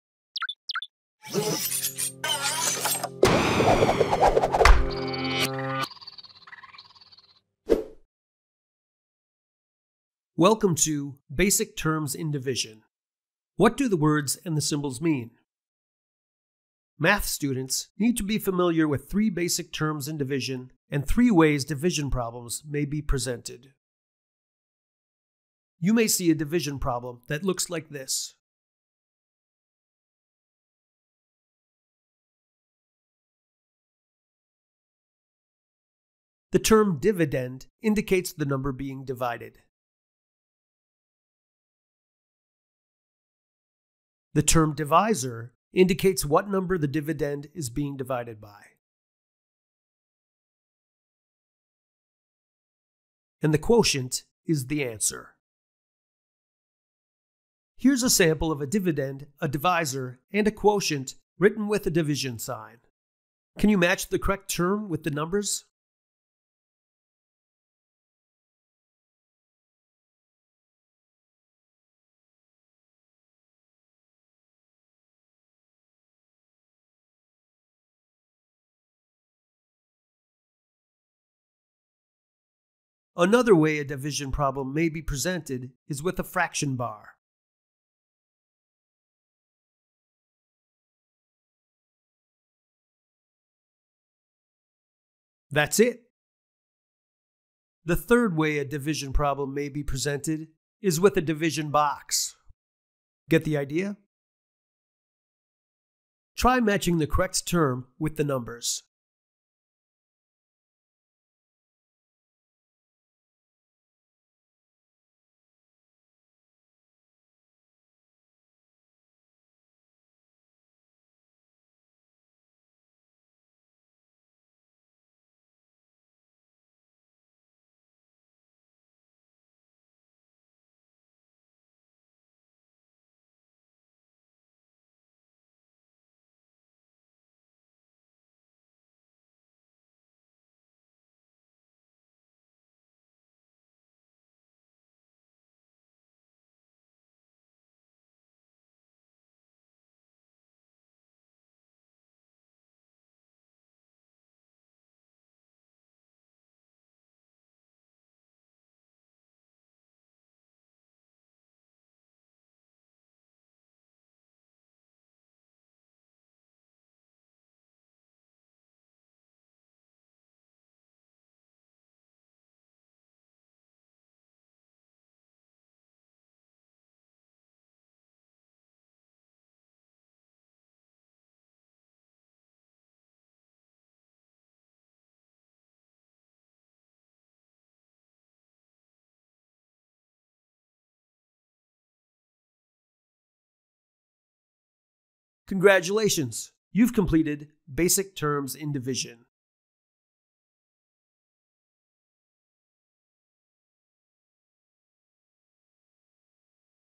Basic Terms in Division
In this interactive object, the learner examines the basic terms used in division.

Credits:
    - Content Author: Barbara Laedtke
    - Video Developer: Justin Stiper